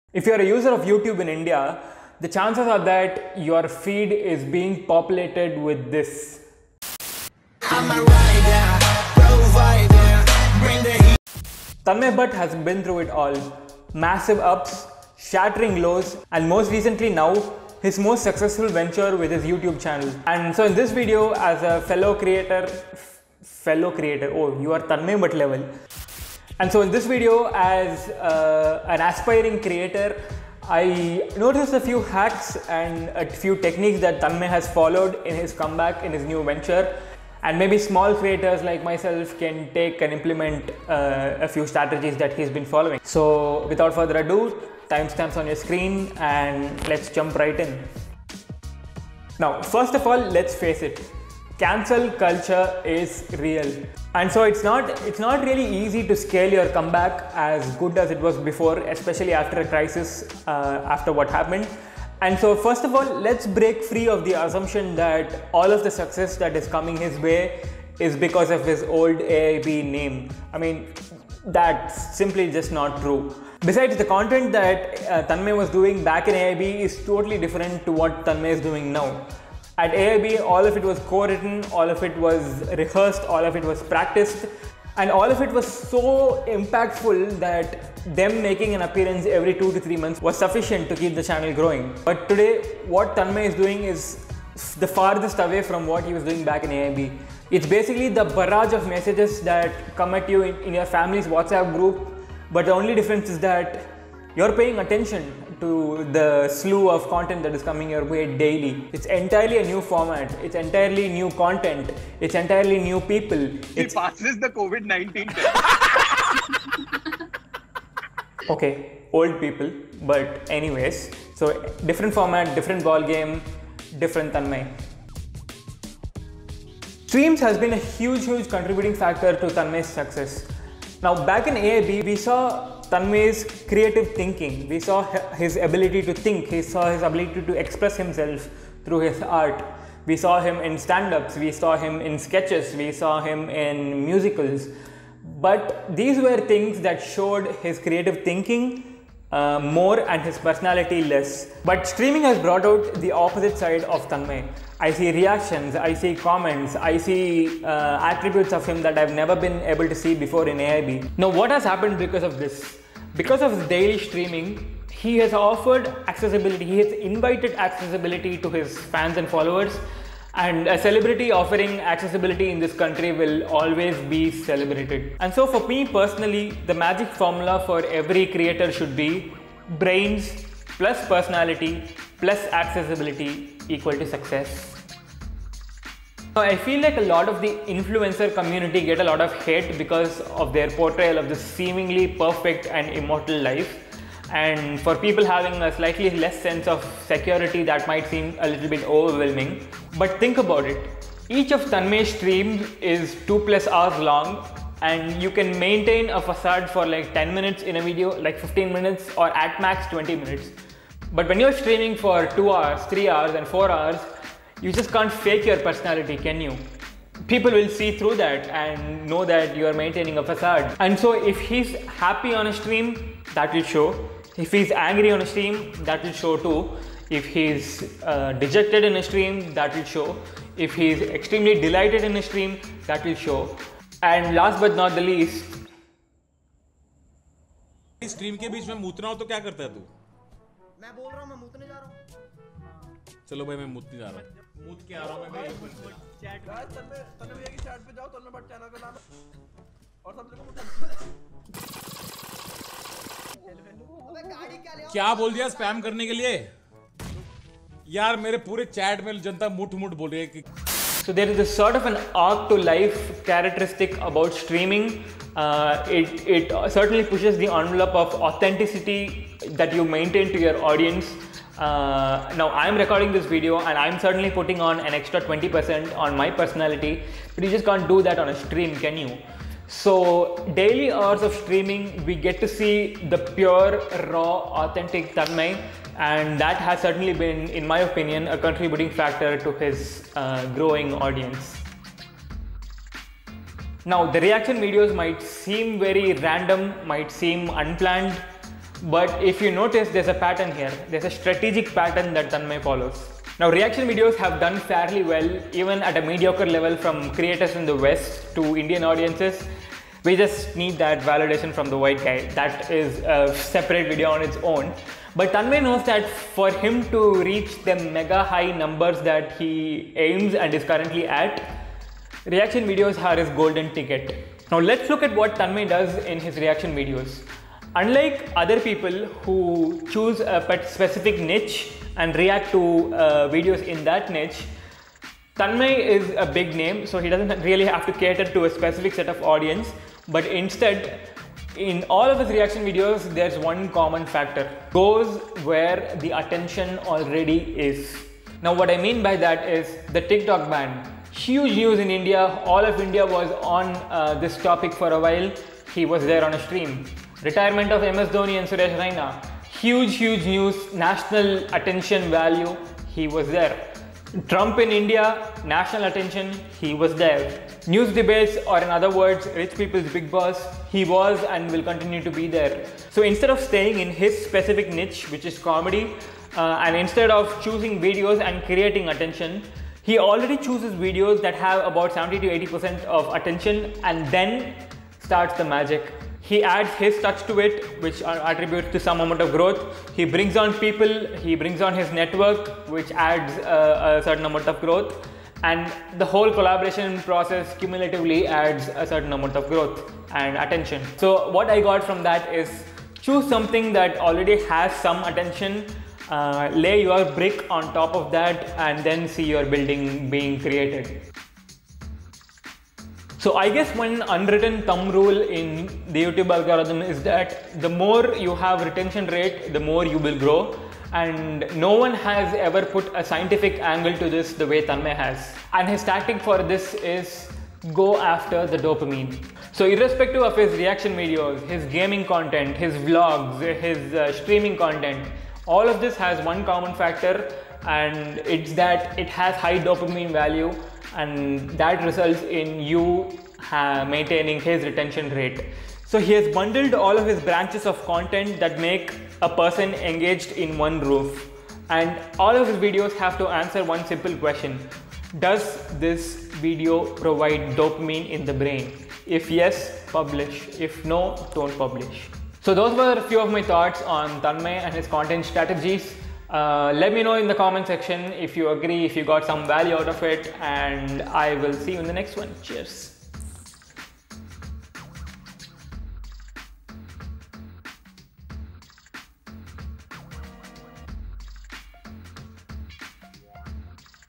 5 PRACTICAL Content Strategies from Tanmay Bhatt That You Can Use IMMEDIATELY to Grow On YouTube
Weather you like him or not, you just can't ignore Tanmay Bhatt. Tanmay Bhatt is a comedian, podacster, content creator and YouTuber. After his devastating downfall from AIB, Tanmay has not only picked himself up but has also gone about his business smartly.

In this video, we take a look at five content hacks that Tanmay uses that you can maybe use a small creator to grow your audience.

Timestamps

00:44 - Intro
00:48 - Chapter 1 - The Comeback Phase
01:13 - Difference in Content between AIB and now
02:05 - Chapter 2 - Streams - The Best Connection Tool
02:31 - Why Streams bring Authenticity to a Creator
04:53 - How Tanmay Strategically Plans His Reaction Videos
07:07 - How Tanmay Maintains High Retention Rate

Arun is a poster designer and a 3D Conceptual artist who has amazing and visually pleasing work. Check him out!